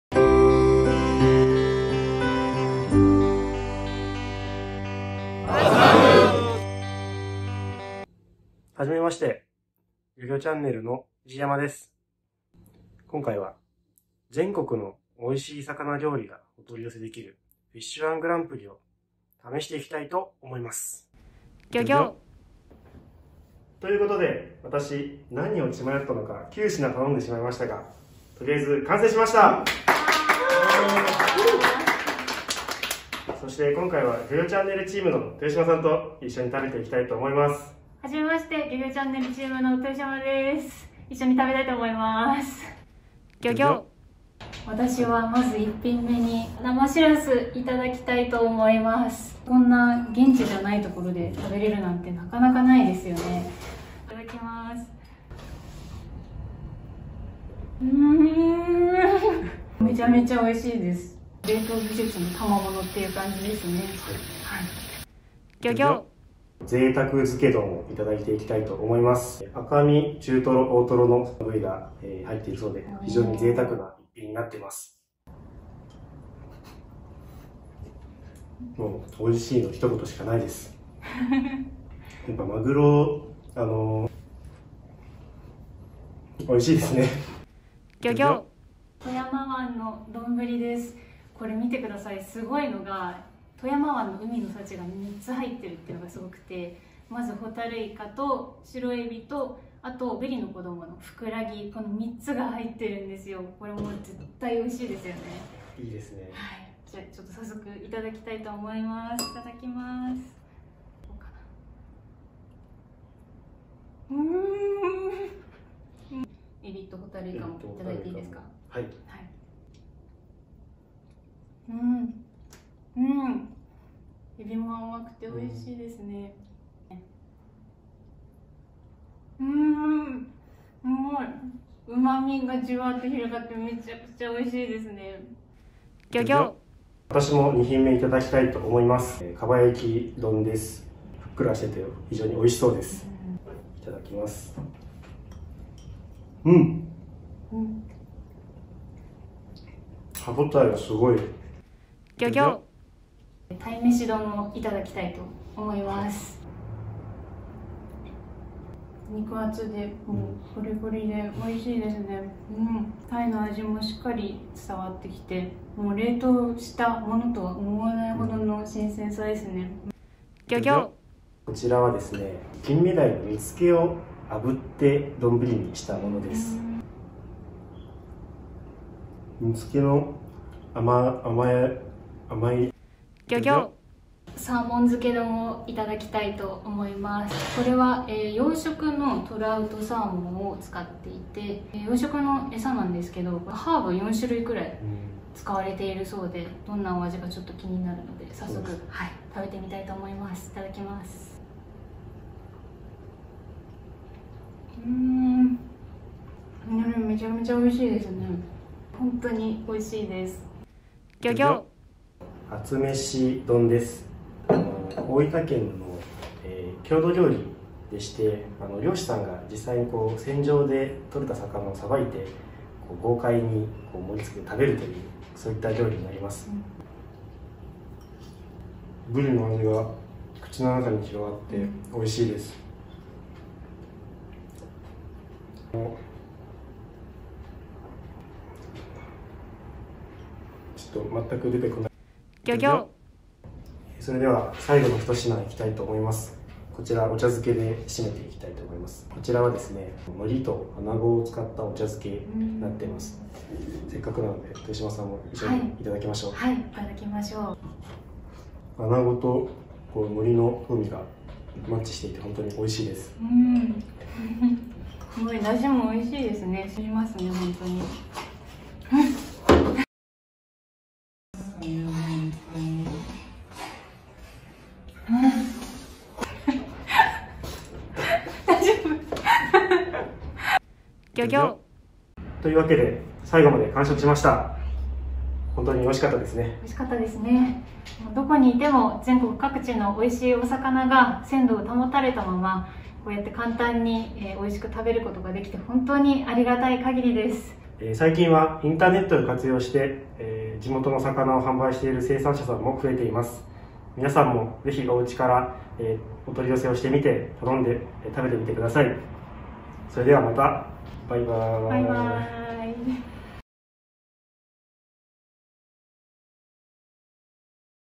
【おうちで漁師飯】全国のおいしい魚を取り寄せてみた！
コロナ禍のおうちご飯の候補に、全国各地のおいしい水産物はいかがですか？最近は、急速冷凍や輸送中の温度管理などの鮮度保持技術によって、新鮮な水産物が全国どこでも食べられるようになっています。
今回は「Fish-1 グランプリ-ONLINE-」にてお魚を注文してみました。漁師が選んだ本当においしい自慢の魚「プライドフィッシュ」や、地魚を使った丼ぶりを自宅でいただきました。
皆さまもぜひおうちでおいしい水産物、食べてくださいね！